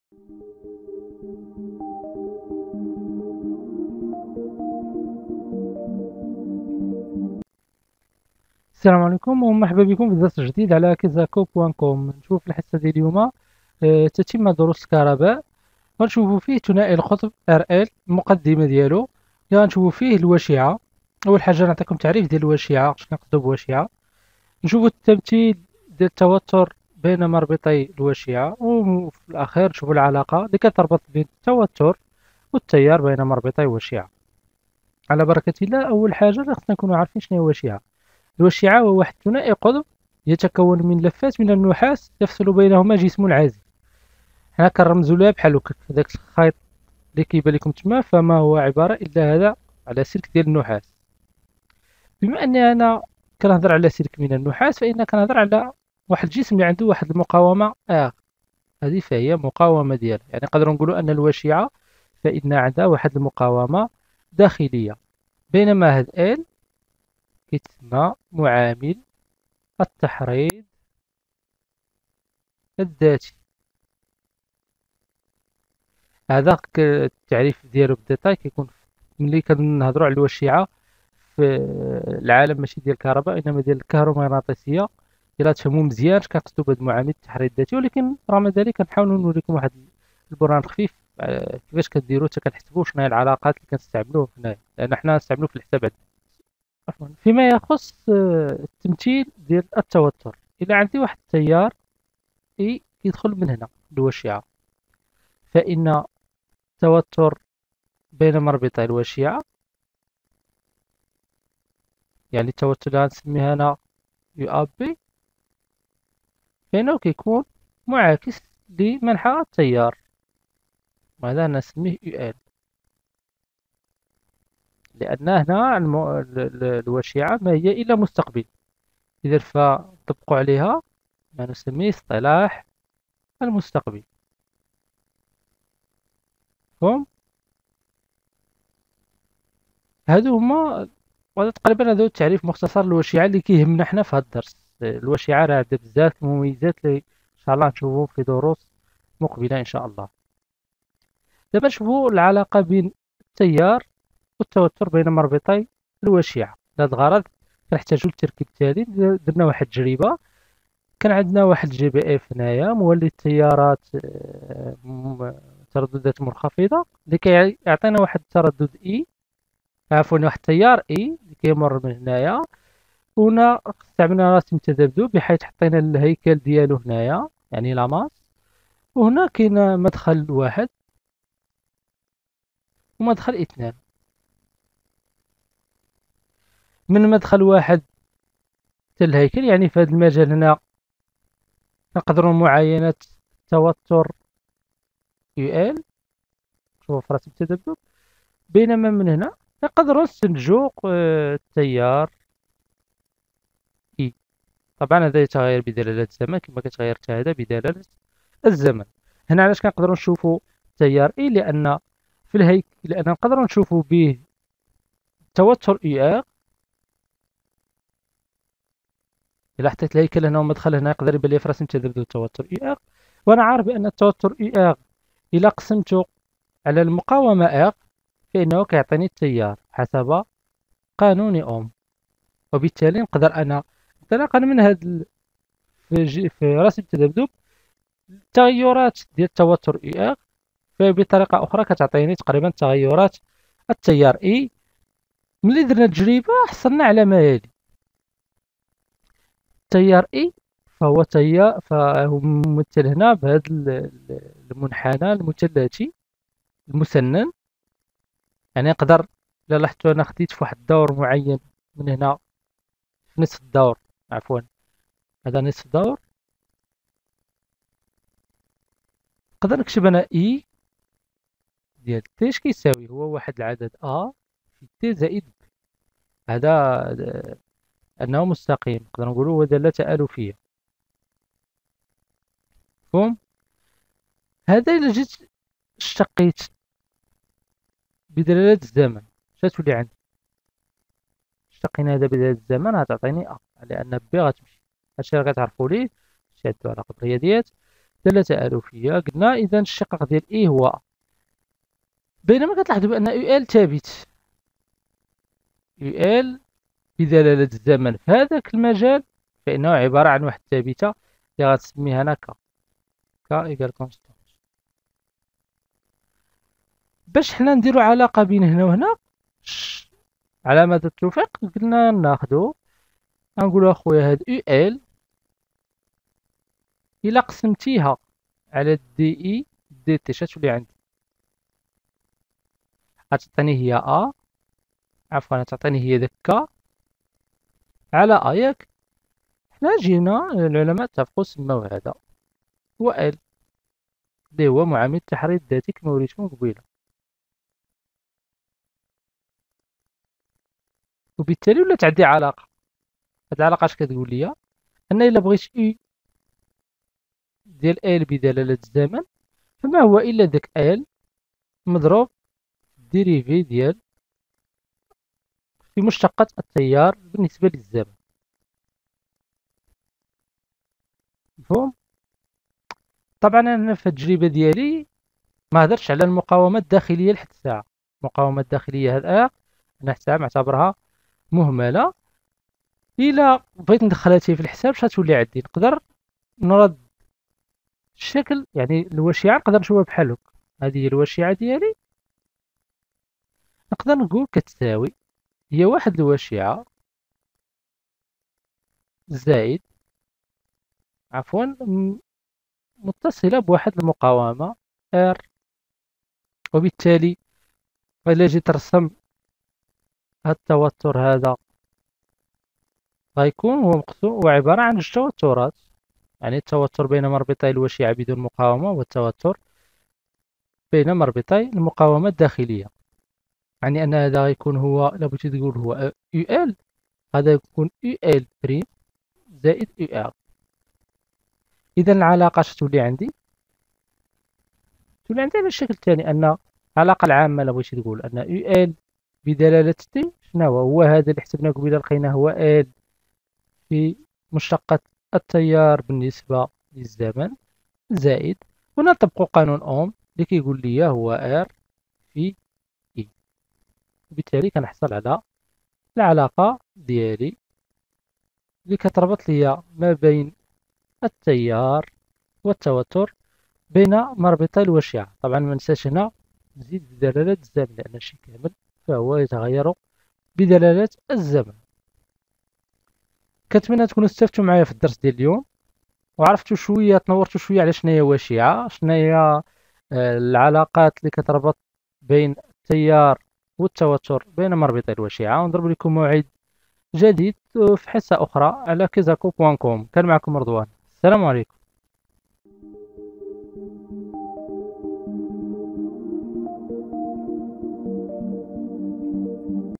السنة ثانية باكالوريا | ثنائي القطب RL - الوشيعة
هذا الفيديو يخص تلاميذ السنة ثانية باكالوريا  و يتضمن شرح شامل لدرس ثنائي القطبRL/الوشيعة
لاشتراك مع قناتنا، اضغطوا على الرابط التالي :